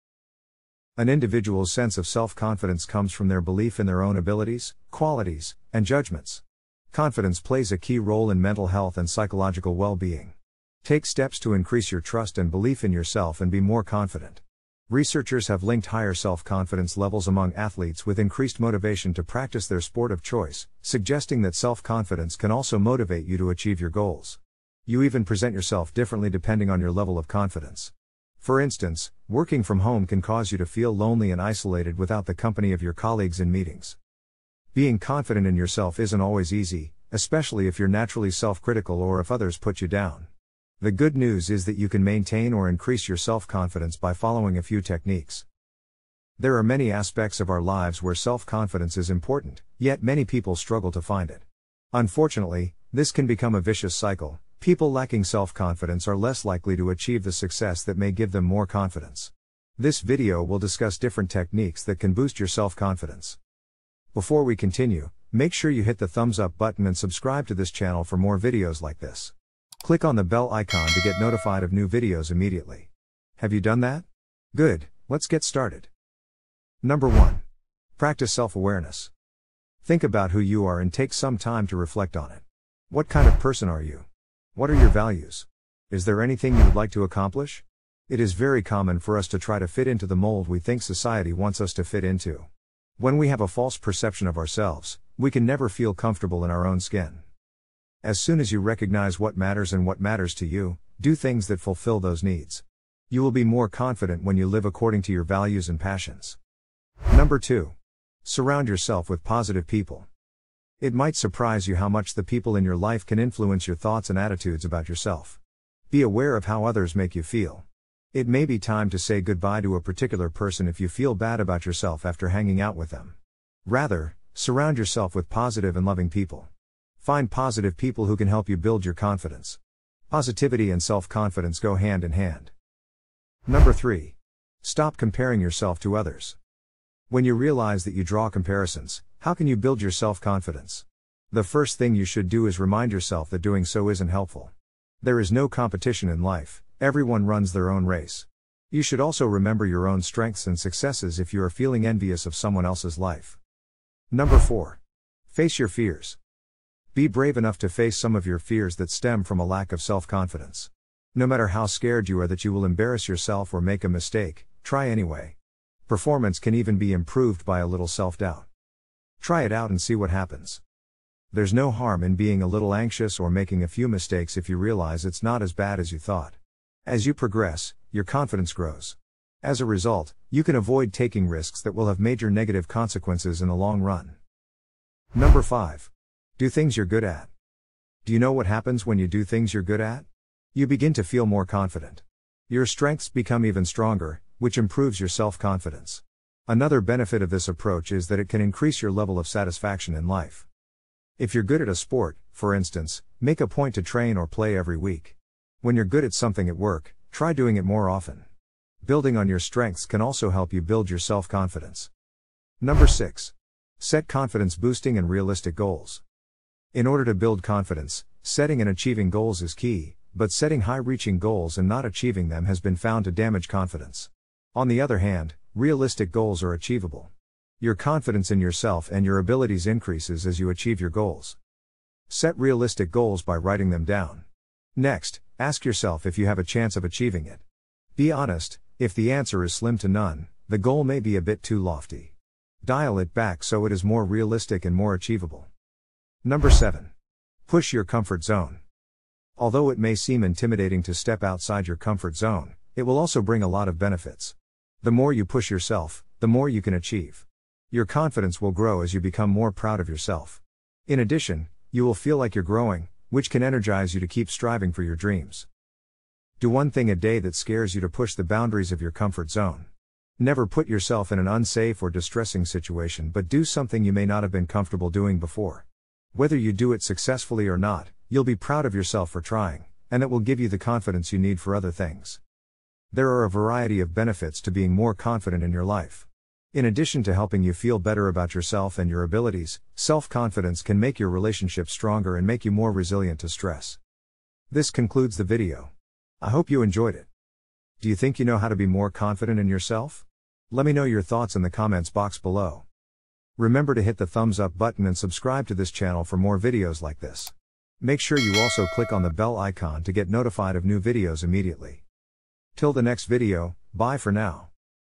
How to build self-confidence/8 Tips to Improve your Self Confidence
An individual's sense of self-confidence comes from their belief in their own abilities, qualities, and judgments. Confidence plays a key role in mental health and psychological well-being. Take steps to increase your trust and belief in yourself and be more confident. Researchers have linked higher self-confidence levels among athletes with increased motivation to practice their sport of choice, suggesting that self-confidence can also motivate you to achieve your goals. You even present yourself differently depending on your level of confidence. For instance, working from home can cause you to feel lonely and isolated without the company of your colleagues in meetings.
Being confident in yourself isn't always easy, especially if you're naturally self-critical or if others put you down. The good news is that you can maintain or increase your self-confidence by following a few techniques@youskills3718